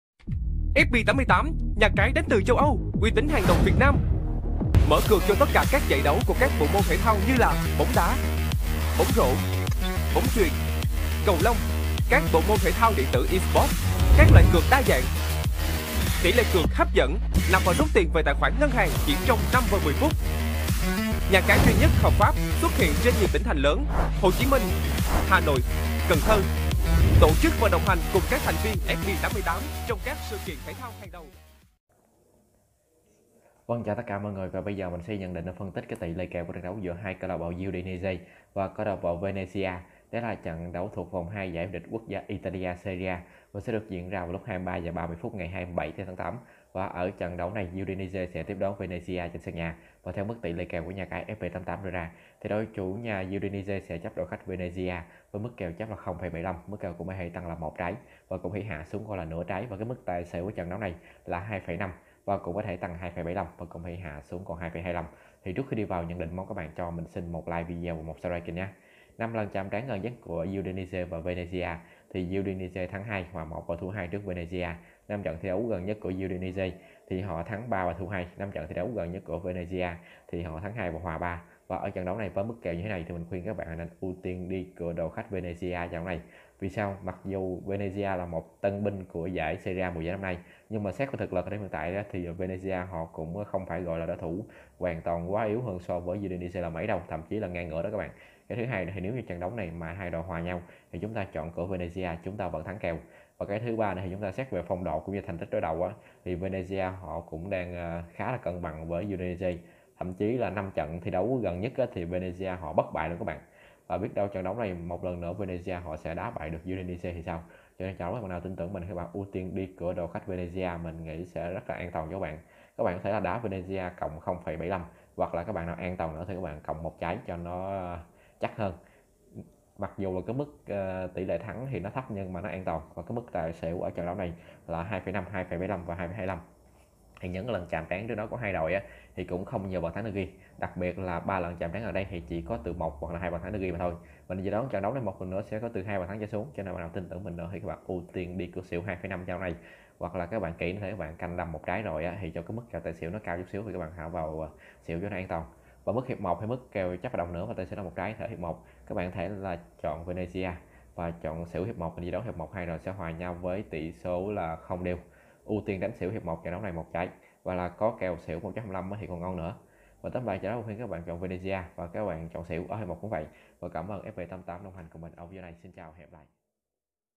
Soi kèo trực tiếp Udinese vs Venezia - 23h30 Ngày 27/8/2021 - vòng 2 Serie A
Lưu ý : không cá cược với mọi hình thức